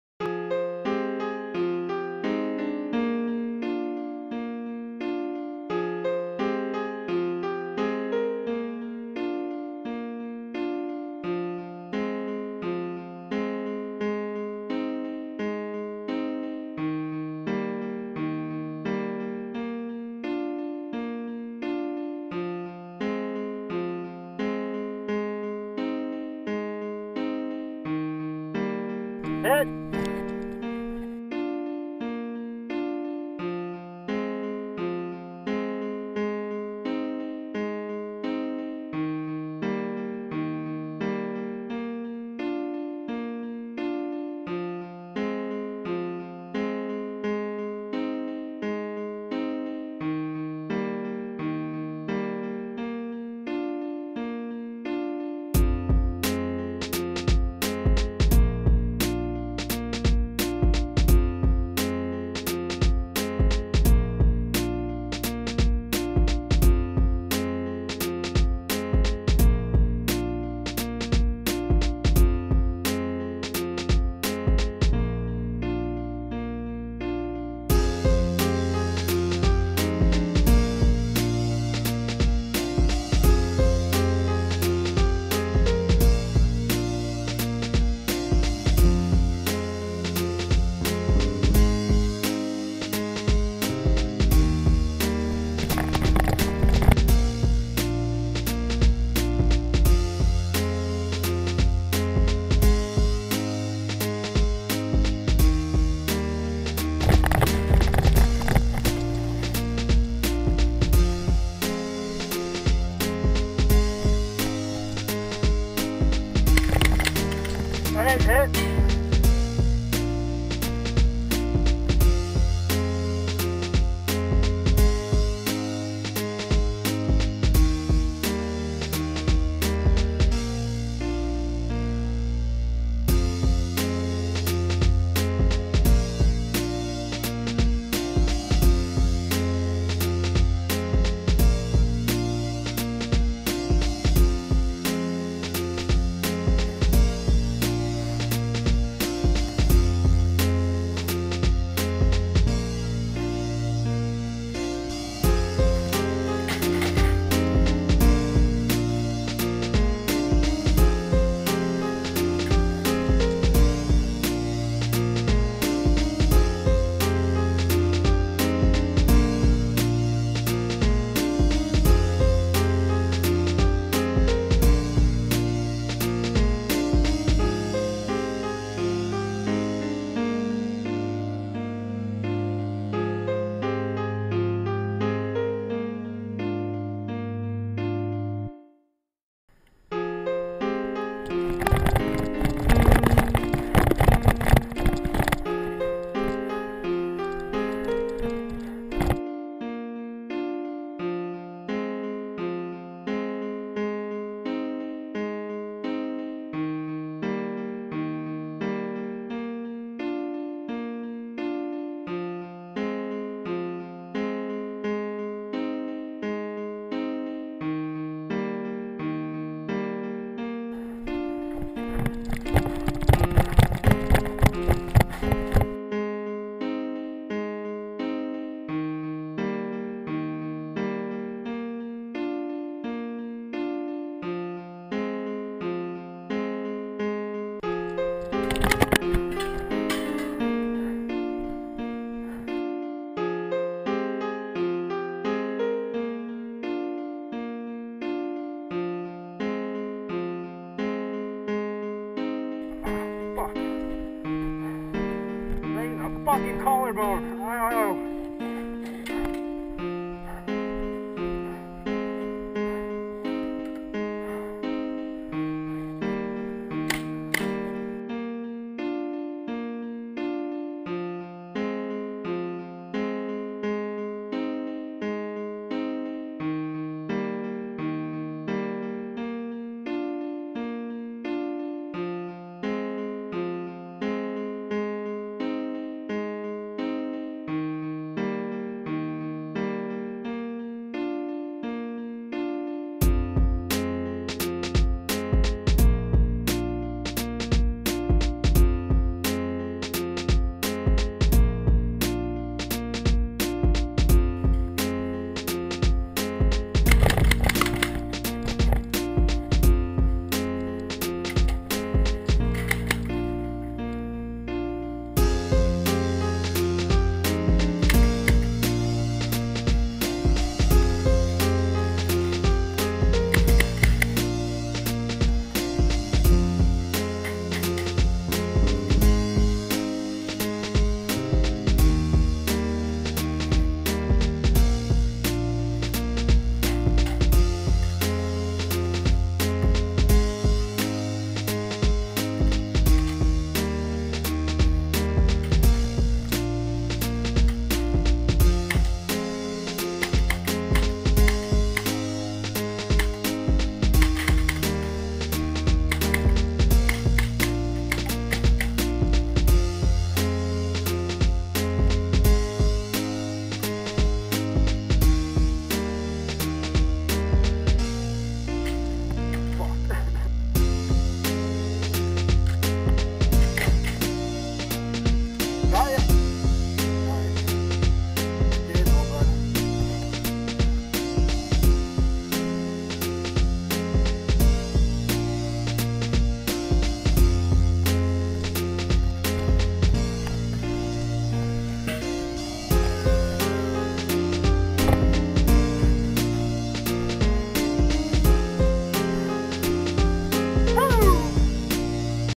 MAG FED "RUN N GUN" AT CQB TORONTO - GOPRO
Great game of 3on3 paintball